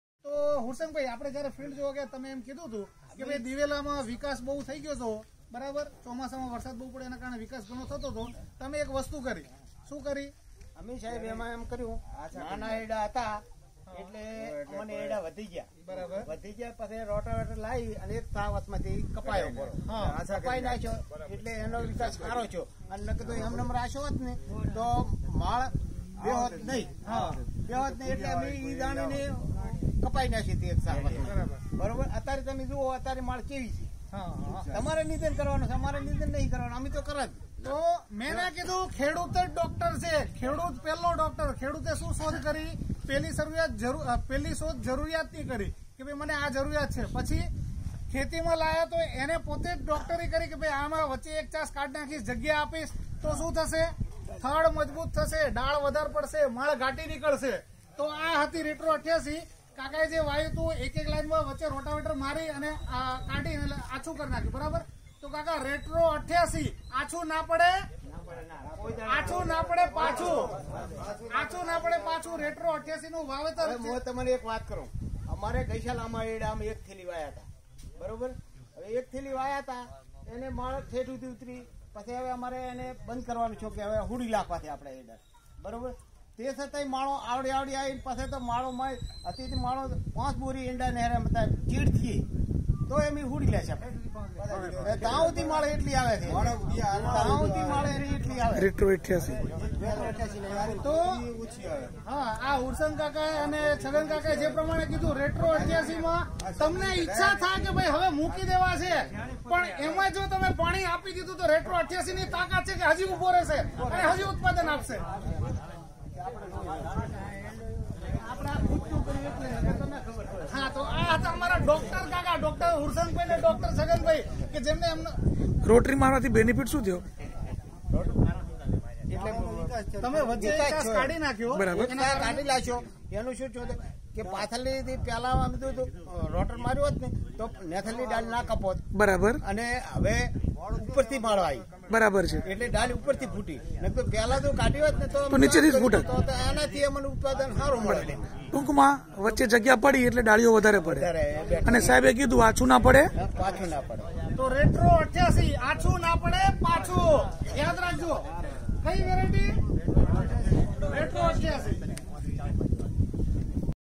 દિવેલાનો અનુભવ રજૂ કરતા ભાભરના ખેડૂત l બનાસકાંઠા l RETRO 88 (Hy Castor) l 9825350245
હા. દિવેલા રીટ્રો ૮૮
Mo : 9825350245